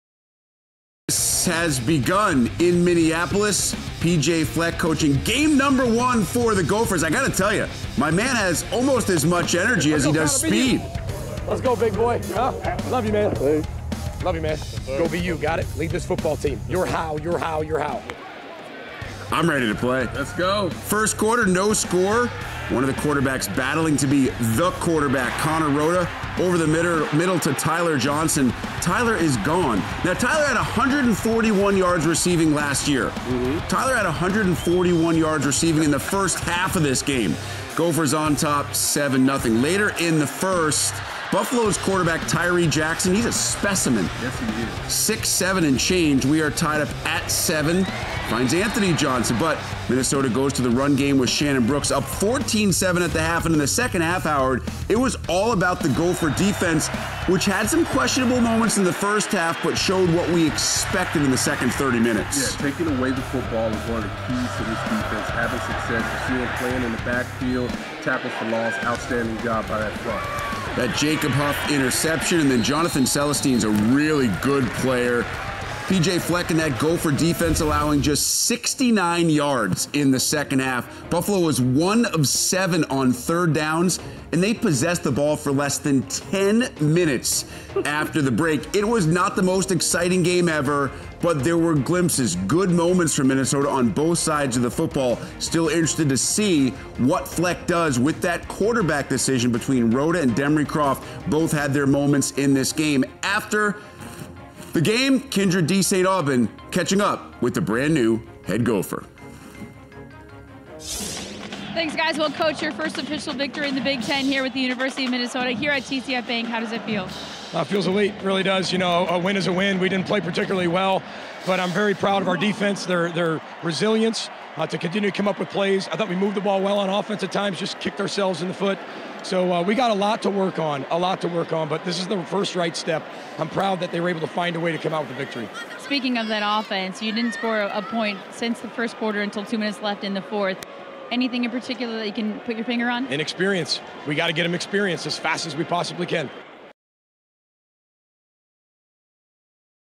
Buffalo at Minnesota - Football Highlights
The Minnesota Golden Gophers open the season with a 17-7 victory over the Buffalo Bulls on Thursday night.